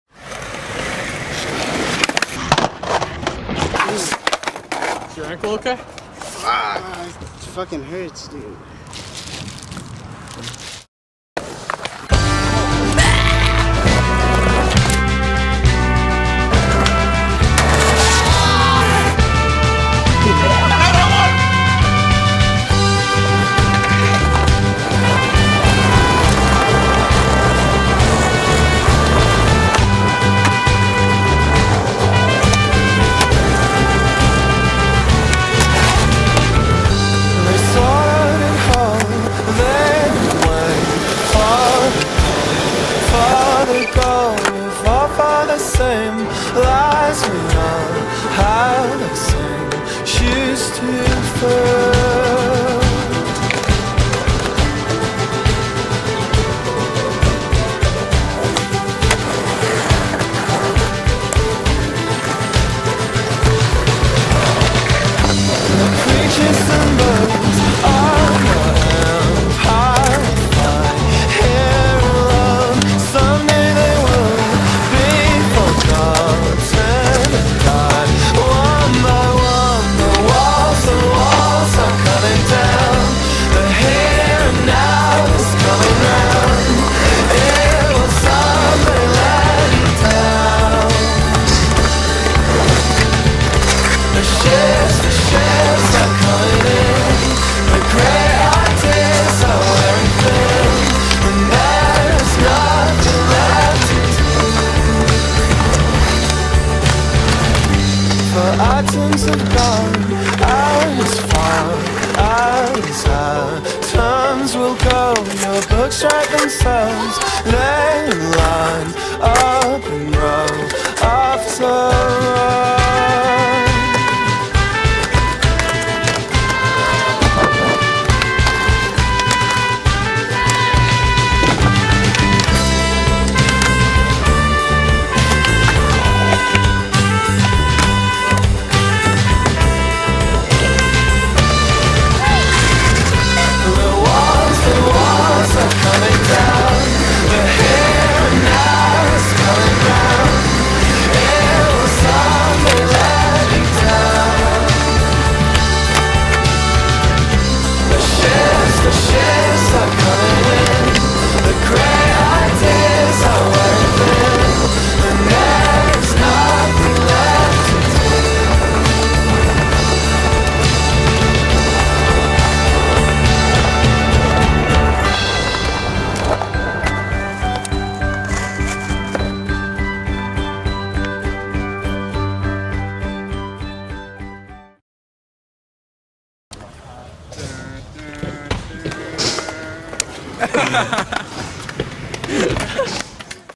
Seattle to San Francisco 2009
fun trip from seattle to sf, i know about the bad fliming